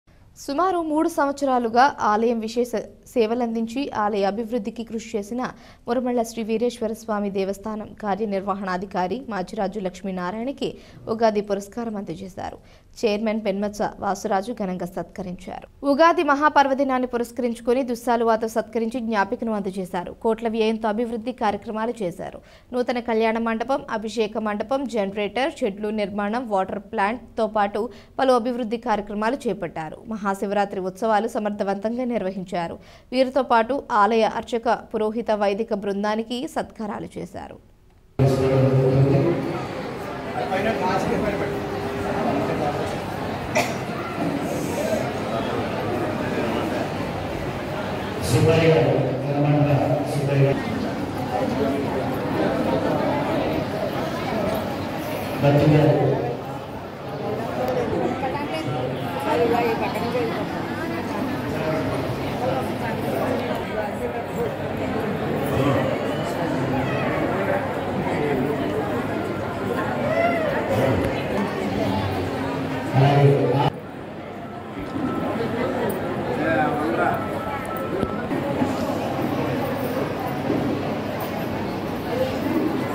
మురమళ్ల శ్రీ వీరేశ్వర స్వామి దేవస్థానం ఆలయం విశేష సేవలందించి ఘనంగా సత్కరించారు.
సుమారు 3 సంవత్సరాలుగా ఆలయం విశేష సేవలందించి ఆలయ అభివృద్ధికి కృషి చేసిన మురమళ్ల శ్రీ వీరేశ్వర స్వామి దేవస్థానం కార్యనిర్వాహణాధికారి మాచిరాజు లక్ష్మీనారాయణ కి ఉగాది పురస్కారం అందజేశారు. చైర్మన్ పెన్మత్స వాసురాజు ఘనంగా సత్కరించారు.

ANC: 2